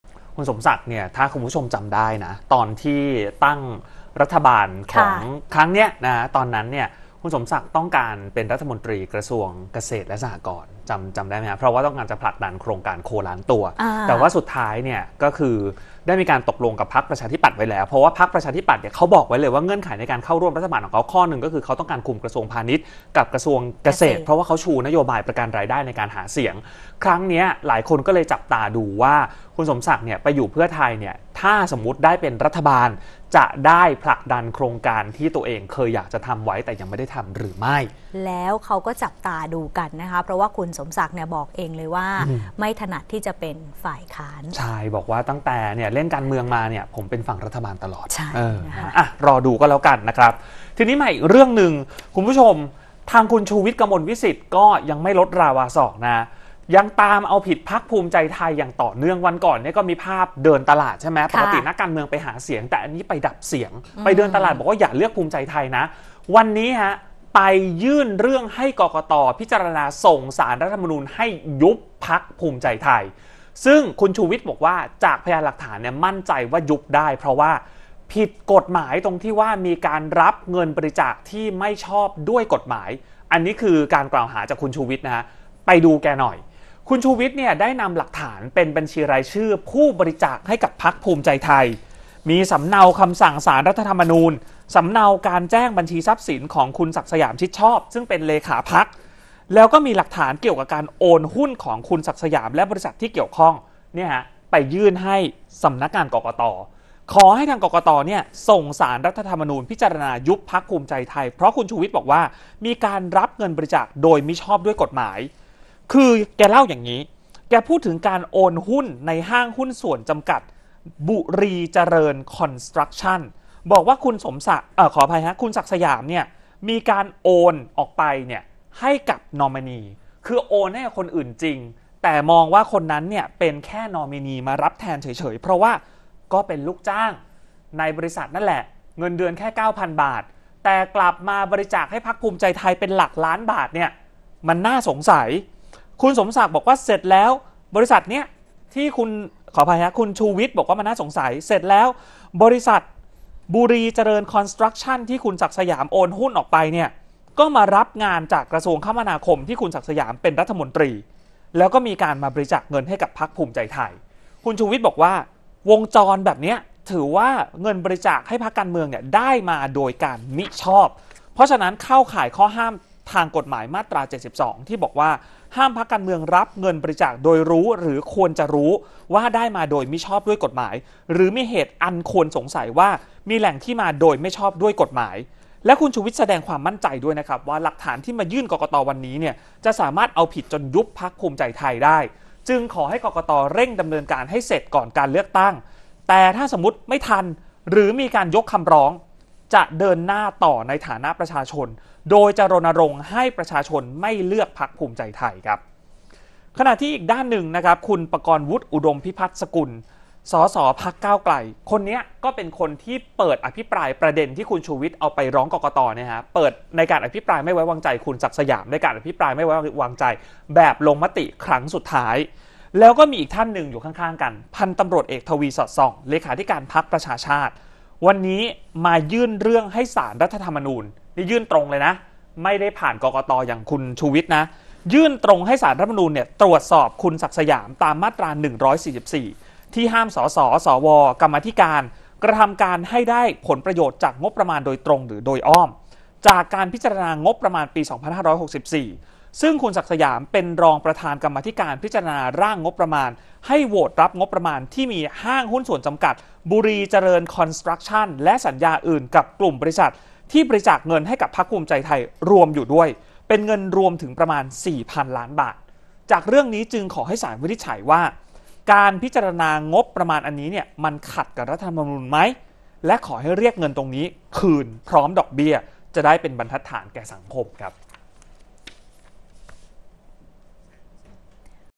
"ชูวิทย์" ยื่นยุบพรรคภูมิใจไทยปมรับเงินบริจาคผิดกฎหมาย ข่าวค่ำ วันที่ 17 มีนาคม 2566
นายชูวิทย์ กมลวิศิษฐ์ ยังตามเอาผิดพรรคภูมิใจไทยต่อเนื่อง โดยวันนี้ไปยื่นเรื่องให้ กกต.พิจารณาส่งศาลรัฐธรรมนูญยุบพรรคภูมิใจไทย กรณีรับเงินบริจาคไม่ชอบด้วยกฎหมาย